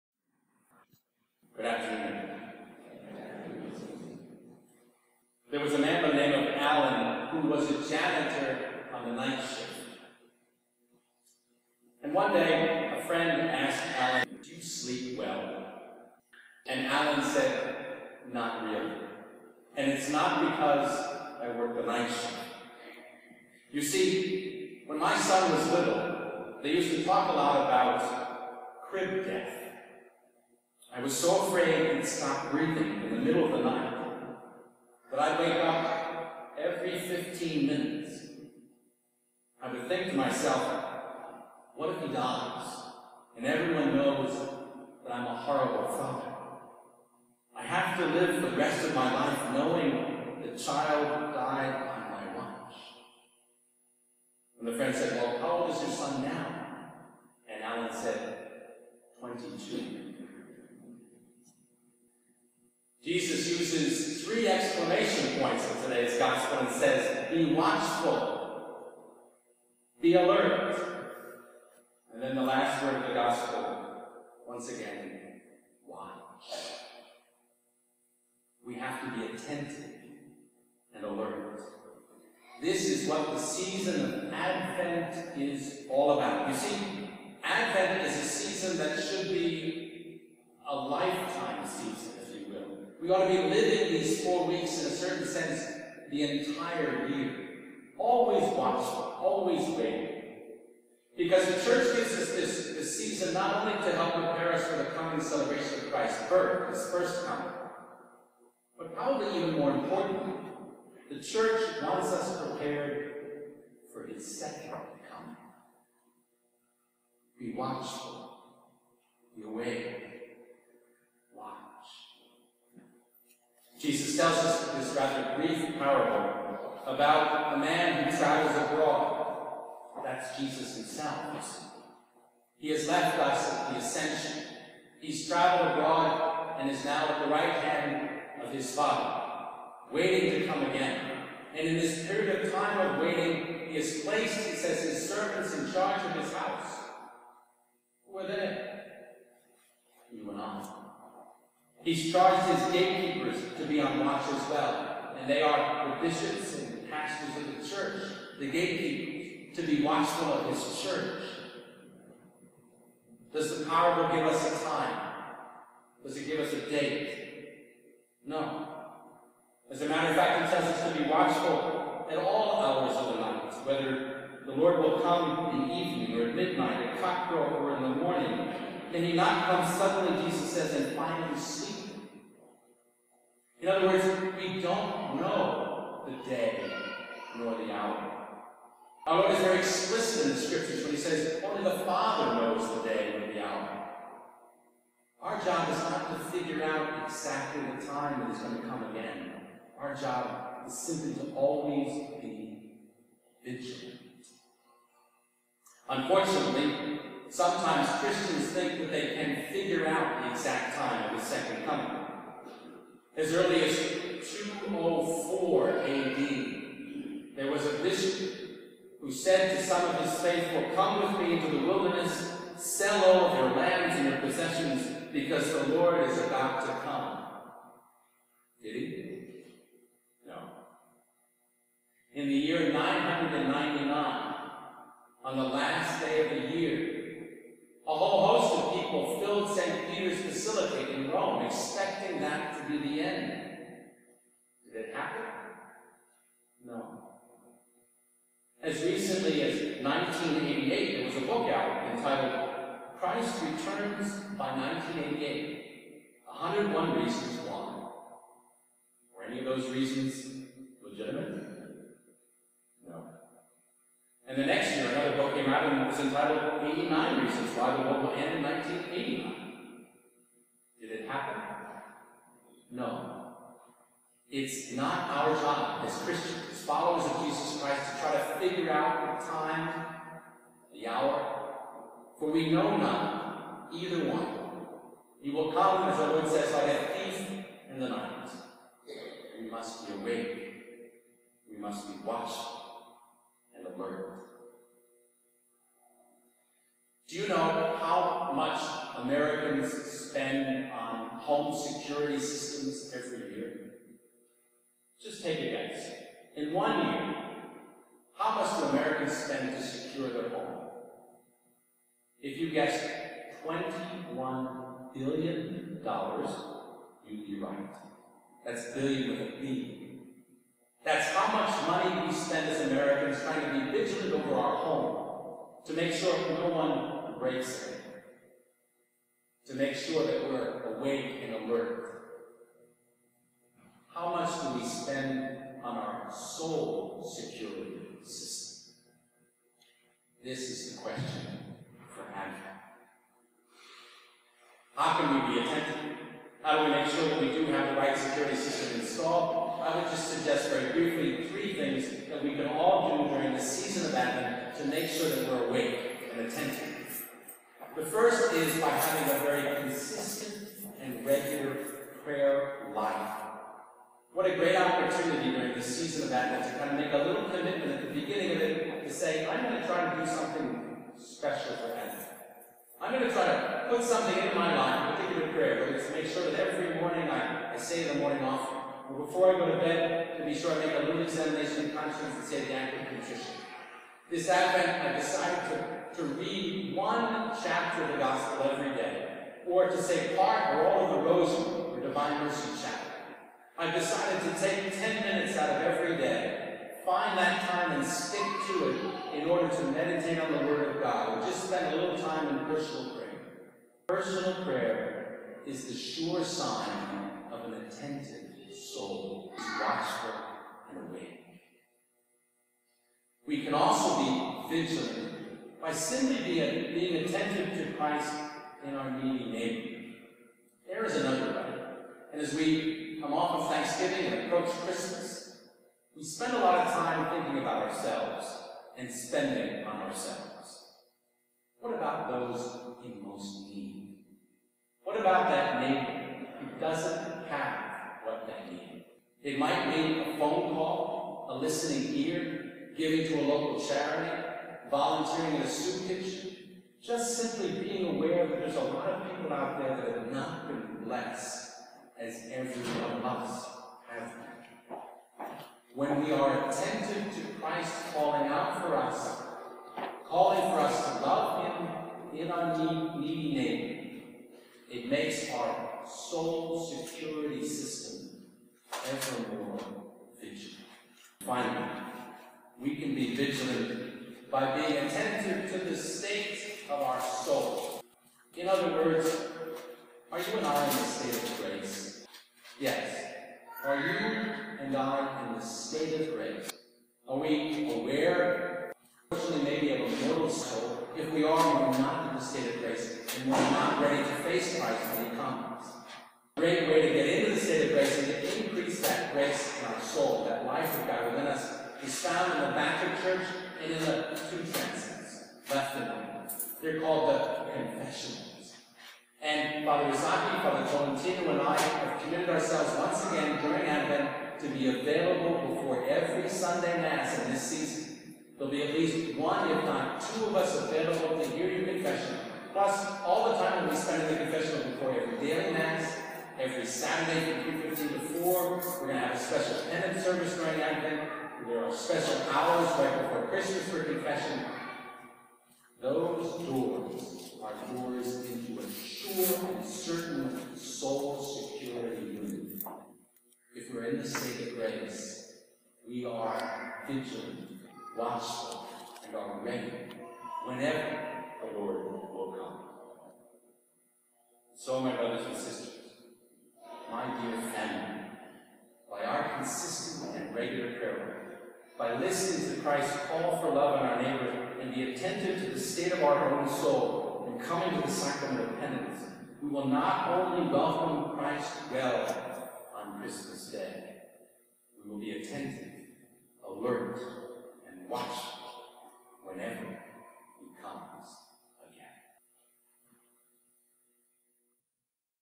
Be Vigilant
Monsignor Andrew Baker sermon 11/30/2014
First Sunday of Advent